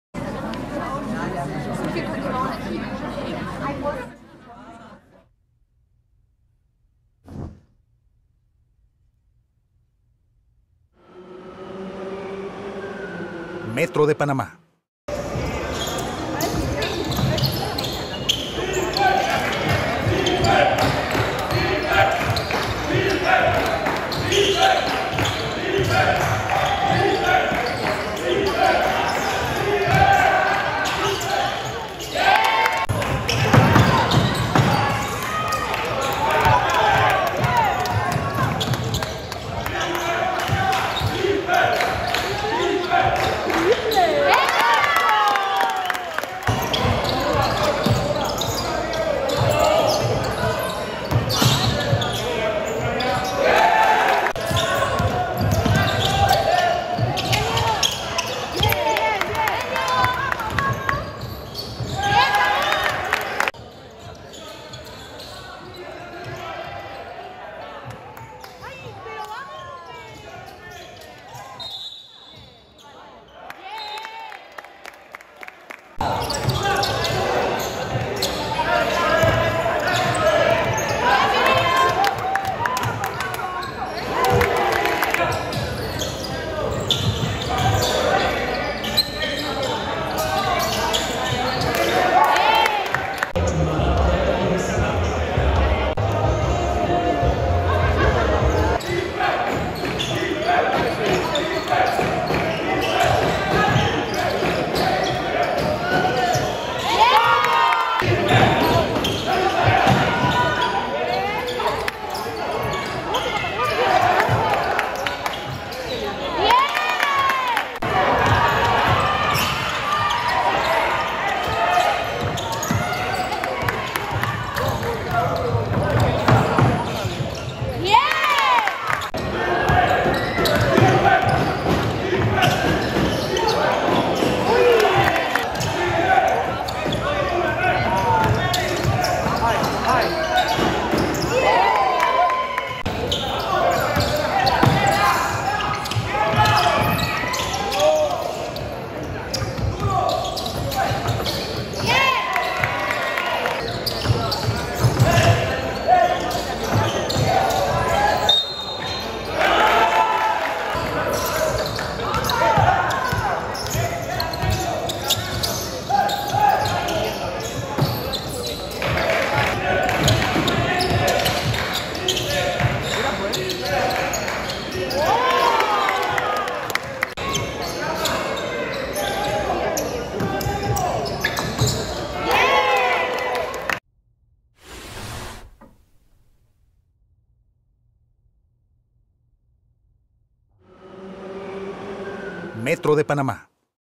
LPB Sub21 | Resumen: Dragones de Don Bosco 82-54 Universitarios de Panamá
Algunas de las mejores jugadas del partido entre Dragones de Don Bosco, y Universitarios de Panamá. Emir Pinillo fue el más destacado por Dragones con 27pts, y 13 rebotes; Joshua Goodridge fue el más destacado por Universitarios con 14pts, y 3 rebotes.